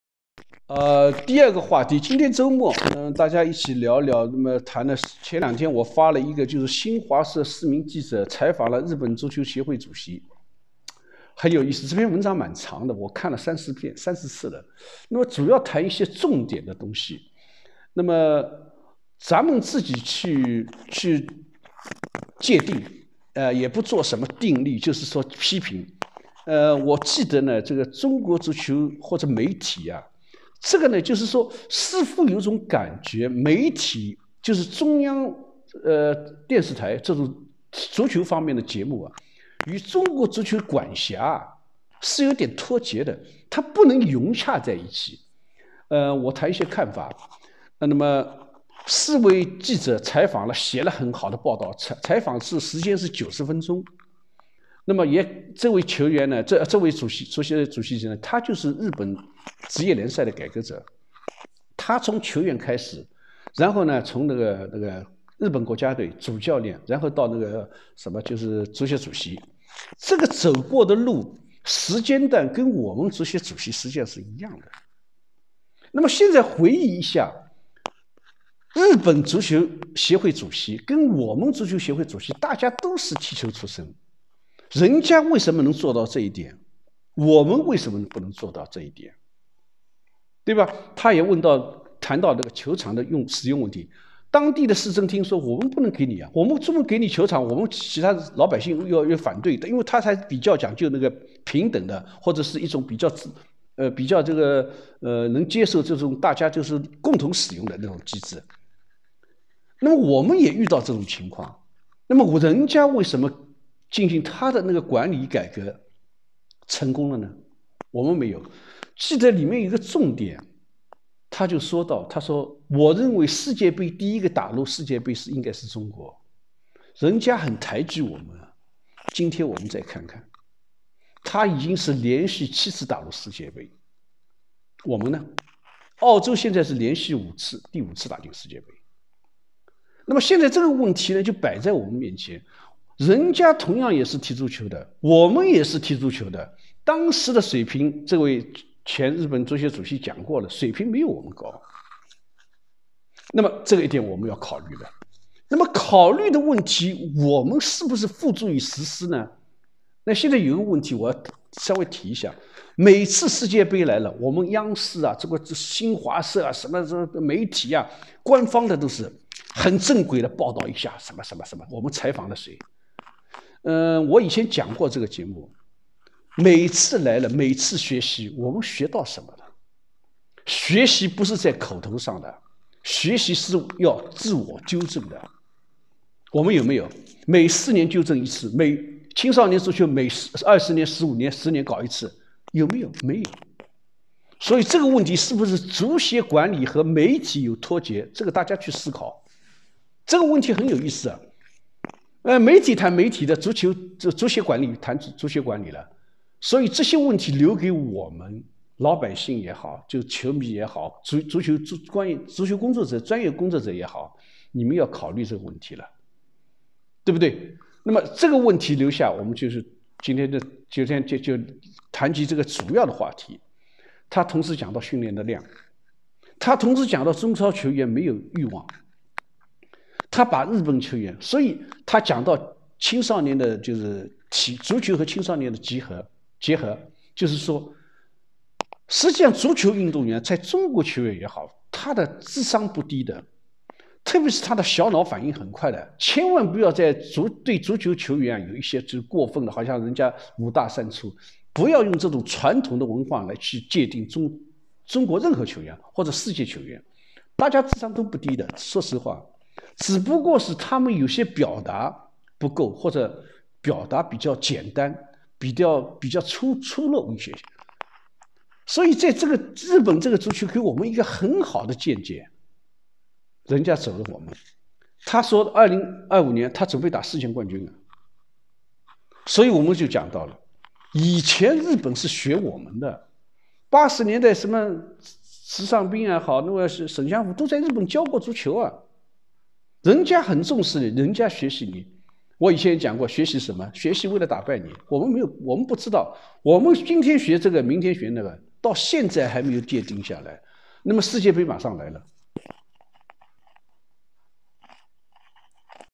2022年7月18日
点评新华社记者采访日本前足协主席的报道文章(1)